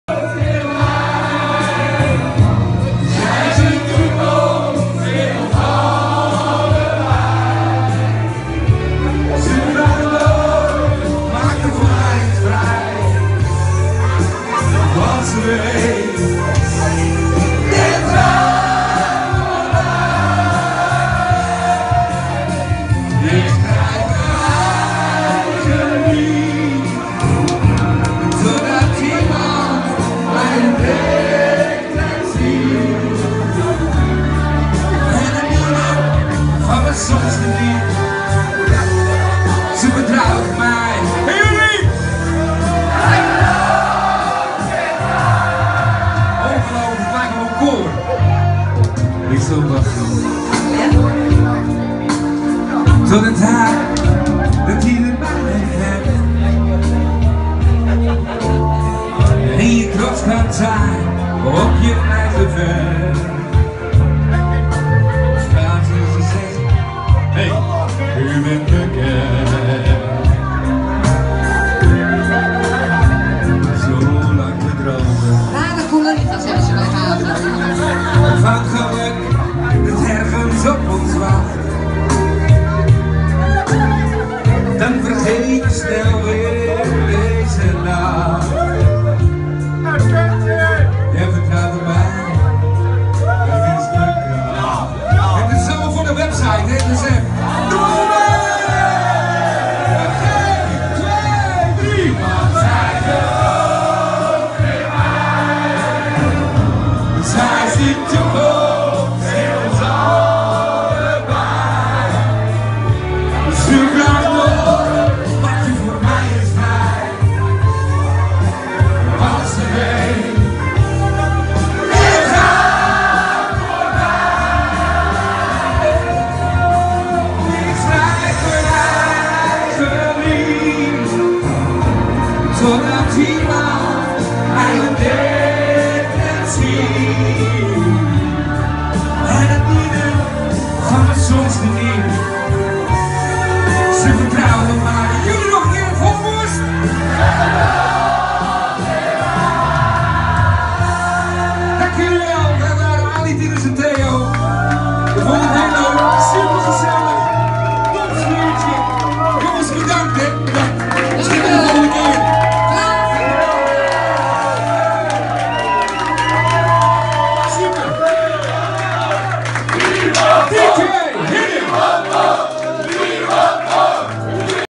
Smartlappentrio Alie, Tinus & Theo
Afgelopen zaterdag in Paleis van de Weemoed in Ansterdan een gezellig bedrijfsfeesie van Hager 20 jarig jubileum.